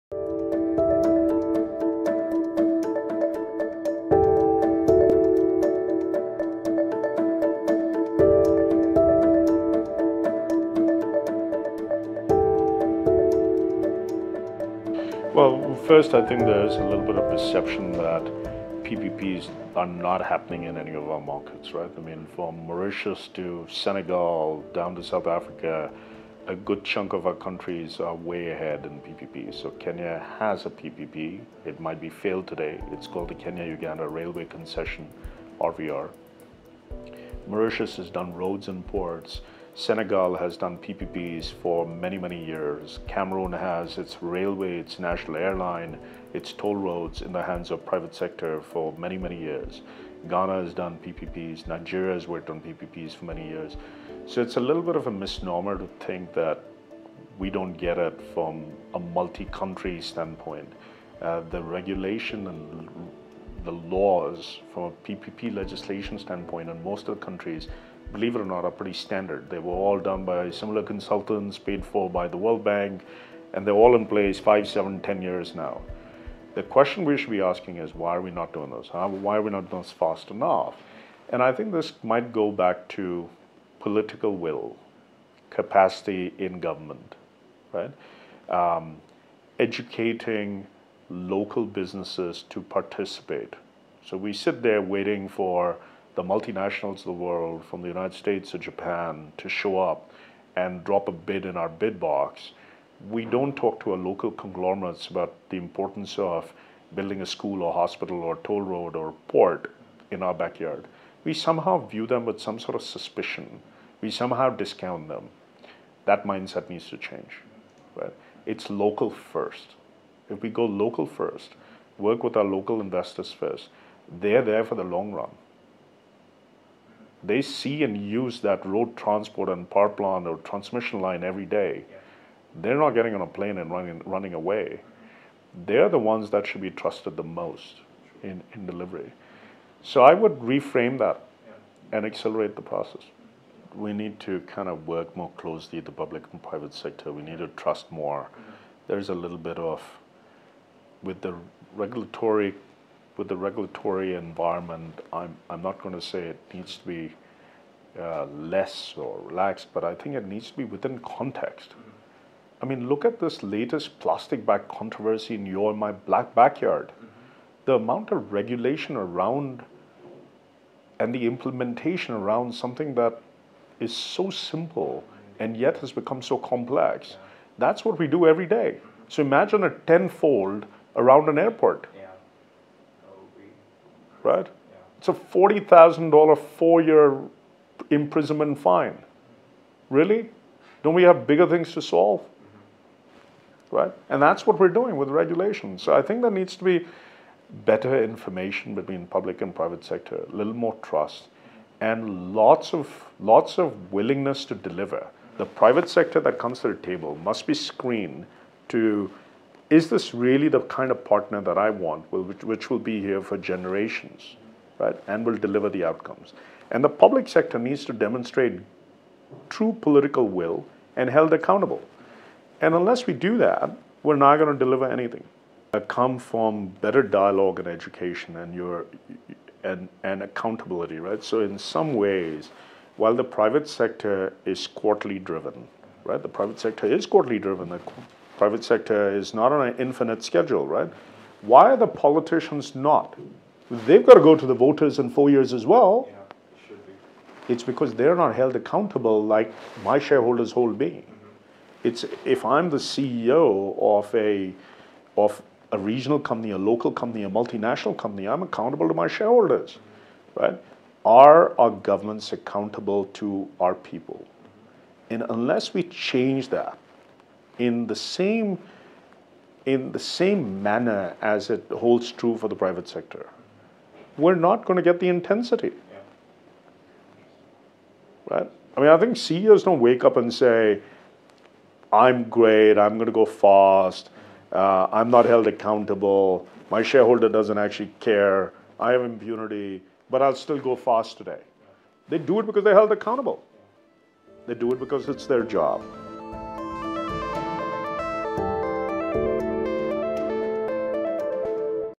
Vishal Agrawal on Perception that Public Private Partnerships are nascent in Africa
There is a misconception that PPP is yet to pick up pace in Africa. Vishal sheds light on some of the projects taking place across Africa.